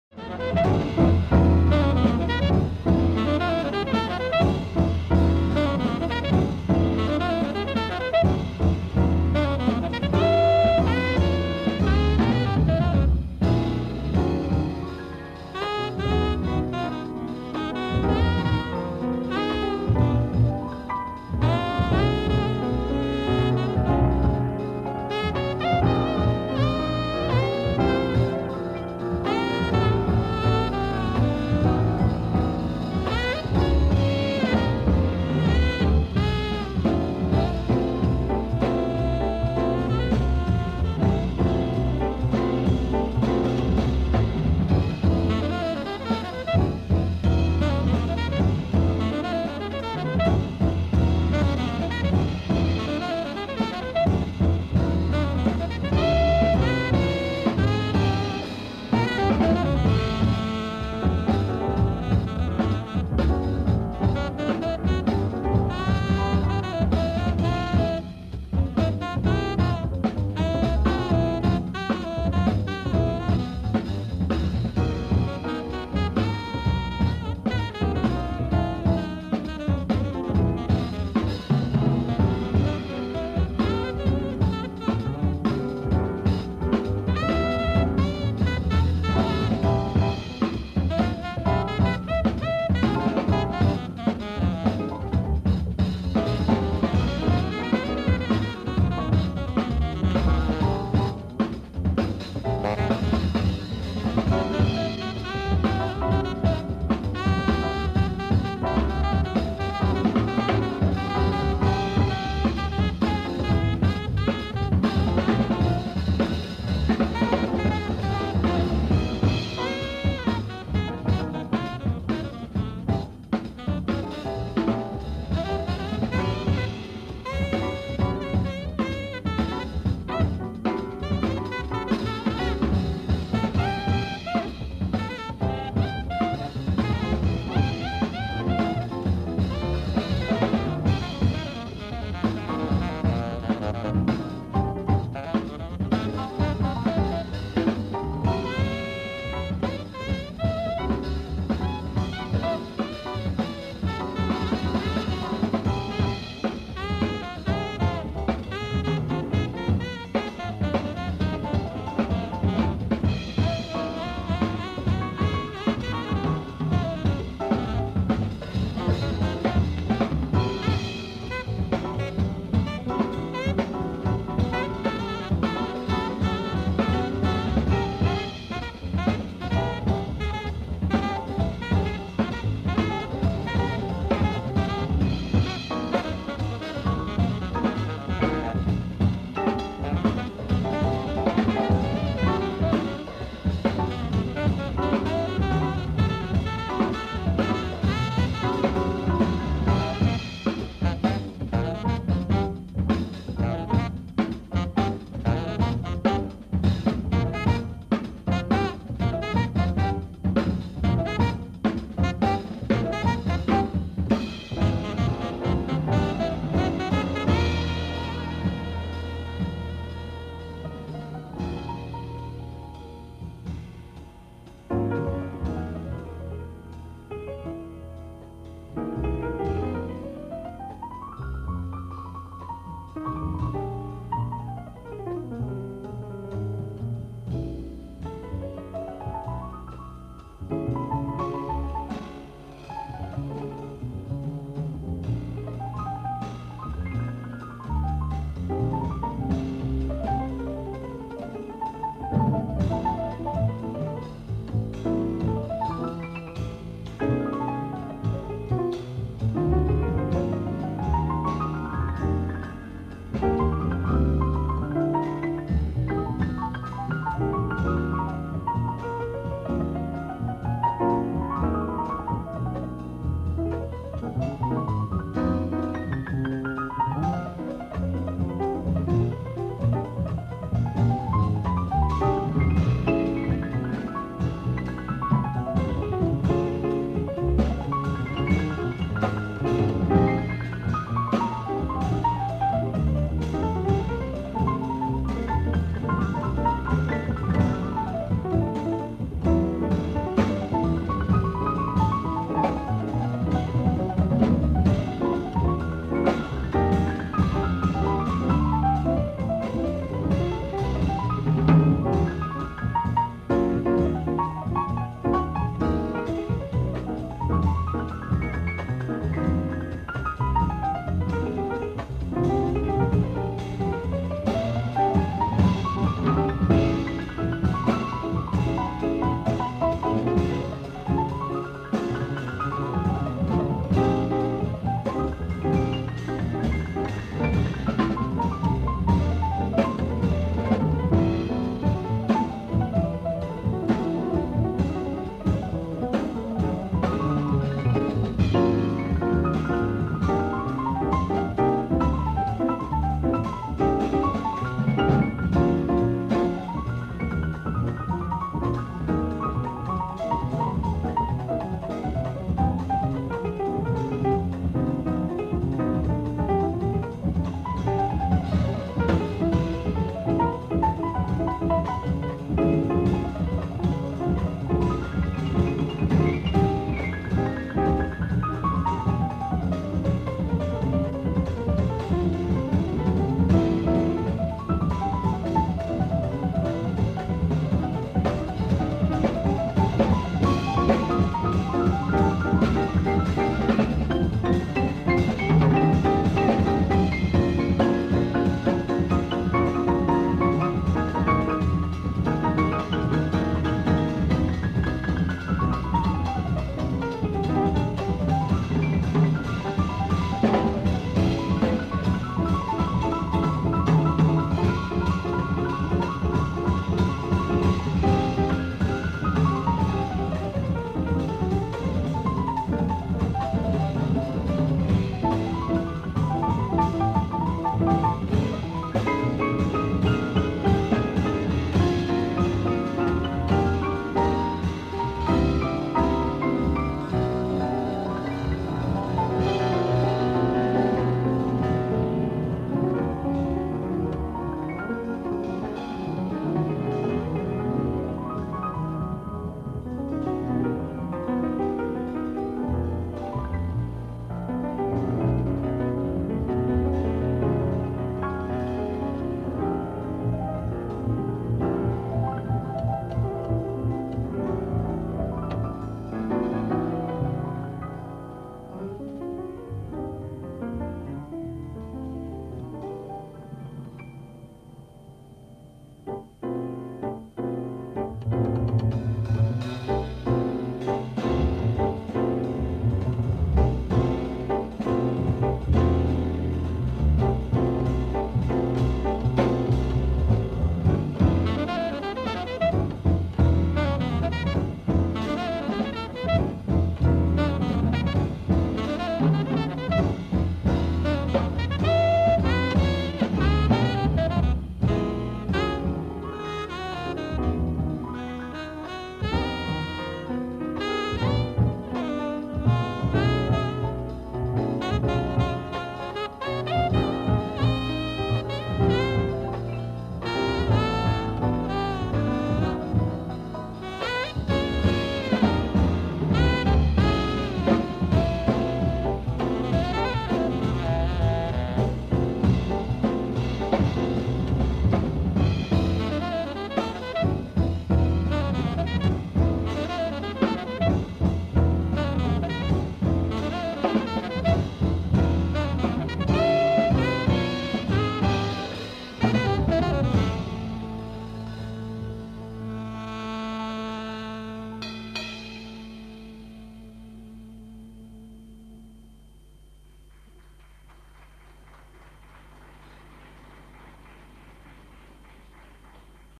Maurizio Giammarco Quartet - Live@ Jazz Tour 1984 - Rapallo
Maurizio Giammarco - Tenor Sax
Stefano Sabatini - Piano
Furio Di Castri - Double Bass
Roberto Gatto - Drums
1984